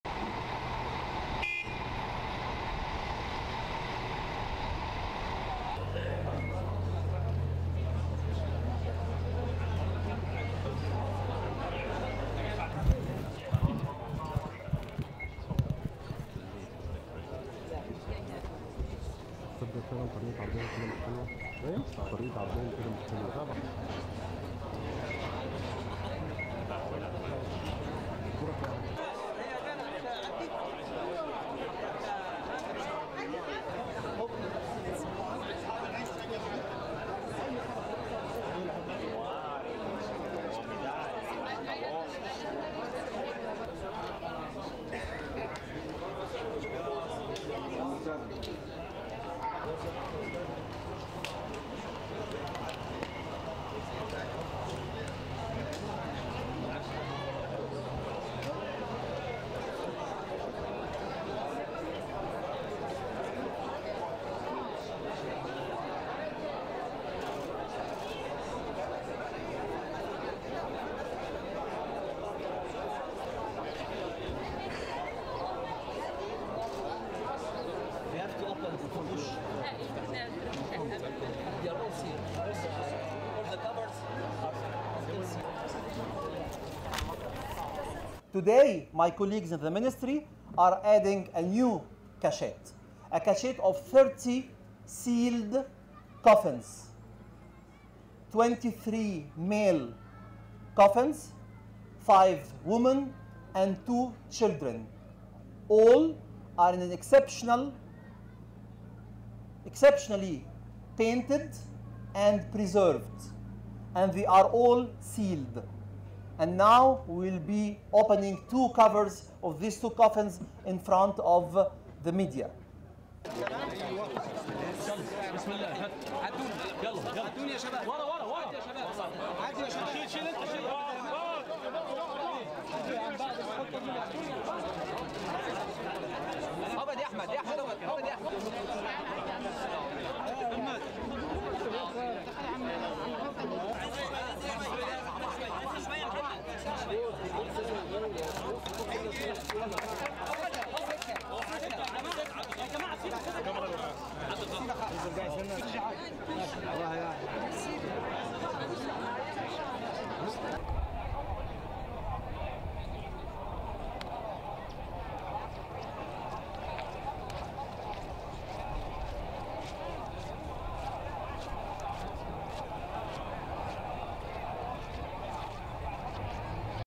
Ancient ancient coffins discovered in Luxor
(20 Oct 2019) ANCIENT ANCIENT COFFINS DISCOVERED IN LUXOR
Egypt's antiquities authority on Saturday unveiled 30 ancient wooden coffins recently discovered in the southern city of Luxor.
 Antiquities minister Khaled el-Anany said that the coffins, with inscriptions and paintings, were found in the Asasif Necropolis on the River Nile's west bank near Luxor.
He said that the coffins were for men, women and children from the 22nd dynasty (945 B.C. 715 B.C.), and had been collected and hidden by a priest for fear of being looted.
The coffins were discovered in two layers, and were all "exceptionally painted and preserved" el-Anany said.
Egypt has sought publicity for its archaeological discoveries in the hopes of reviving its tourism sector, which was badly hit by the turmoil following the 2011 uprising.